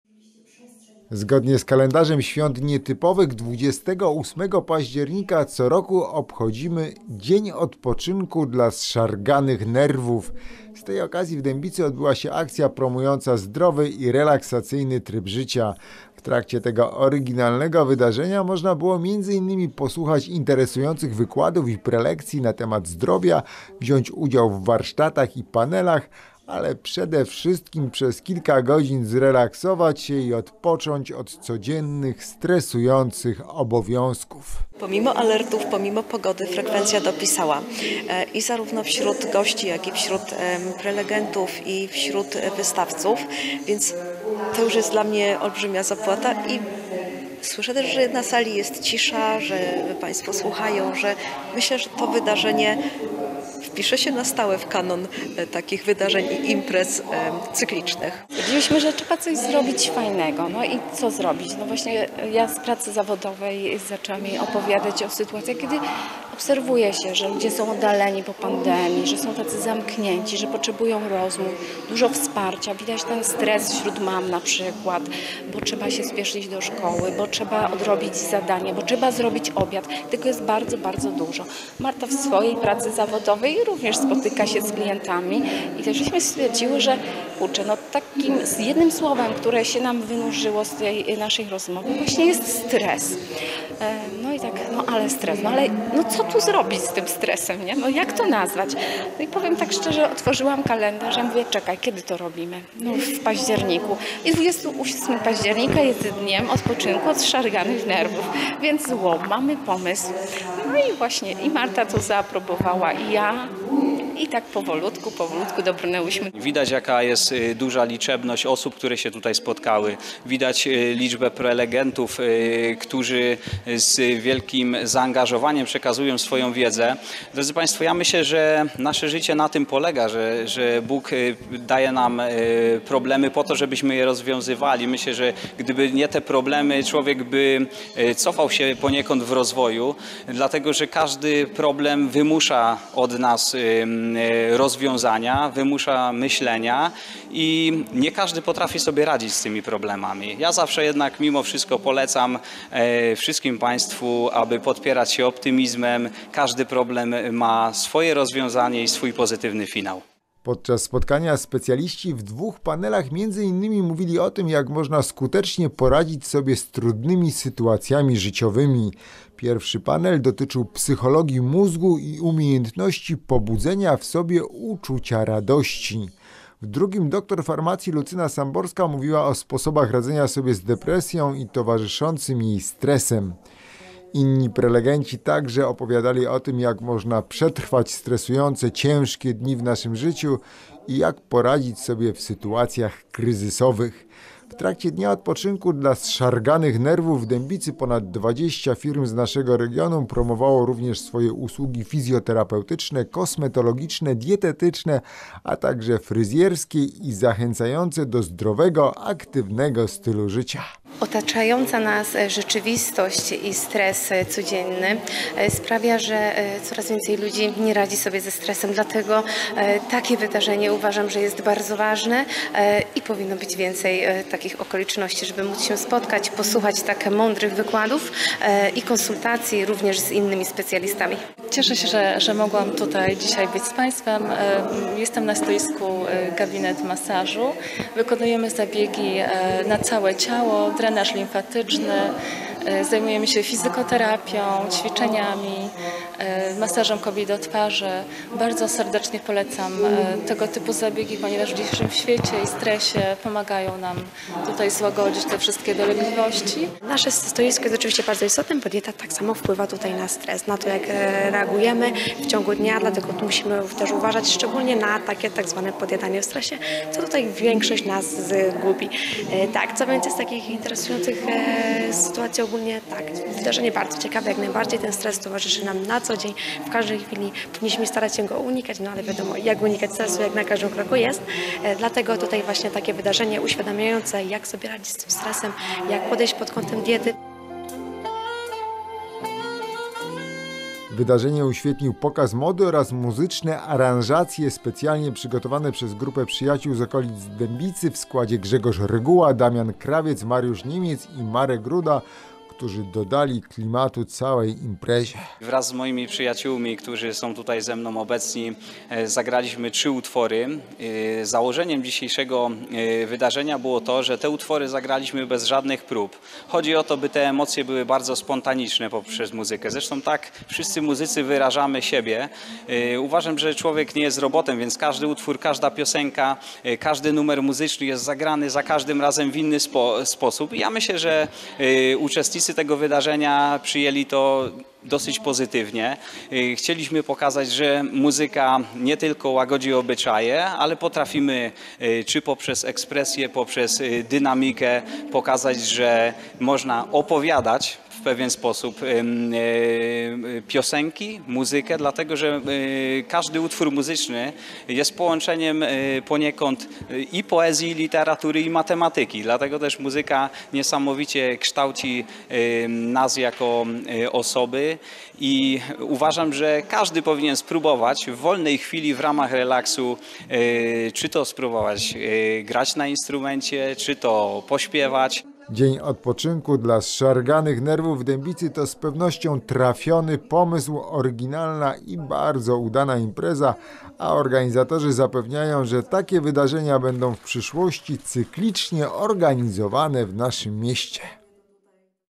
„Dzień Odpoczynku dla Zszarganych Nerwów” w Dębicy
Zgodnie z kalendarzem świąt nietypowych 28 października co roku obchodzimy  „Dzień Odpoczynku dla Zszarganych Nerwów” J .

Z tej okazji w Dębicy odbyła się akcja promująca zdrowy i relaksacyjny tryb życia.

W trakcie tego oryginalnego wydarzenia można było między innymi posłuchać interesujących wykładów i prelekcji na temat zdrowia, wziąć udział w warsztatach i panelach, ale przede wszystkim przez kilka godzin zrelaksować się
i odpocząć od codziennych, stresujących obowiązków.

Podczas spotkania specjaliści  w dwóch panelach między innymi mówili o tym: „jak można skutecznie poradzić  sobie z trudnymi sytuacjami życiowymi".

Pierwszy panel  dotyczył  „psychologii mózgu i umiejętności pobudzenia w sobie uczucia radości”.

W drugim dr farmacji Lucyna Samborska mówiła o „sposobach radzenia sobie z depresją i towarzyszącym jej stresem”.

Inni prelegenci także opowiadali o tym „jak można przetrwać stresujące, ciężkie dni w naszym życiu
i jak poradzić sobie w sytuacjach kryzysowych”.

W trakcie „Dnia Odpoczynku dla Zszarganych Nerwów” w Dębicy ponad 20 firm z naszego regionu promowało  również swoje usługi fizjoterapeutyczne, kosmetologiczne, dietetyczne,  a także fryzjerskie i zachęcające do zdrowego, aktywnego stylu życia.

Wydarzenie uświetnił pokaz mody oraz muzyczne aranżacje, specjalnie przygotowane przez „Grupę Przyjaciół z okolic Dębicy” w składzie:  Grzegorz Reguła, Damian Krawiec, Mariusz Niemiec i Marek Ruda, którzy dodali klimatu całej imprezie.

„Dzień Odpoczynku dla Zszarganych Nerwów” w Dębicy to z pewnością trafiony pomysł, oryginalna i bardzo udana impreza, organizatorzy zapewniają, że takie wydarzenia  będą w przyszłości cyklicznie organizowane w naszym mieście.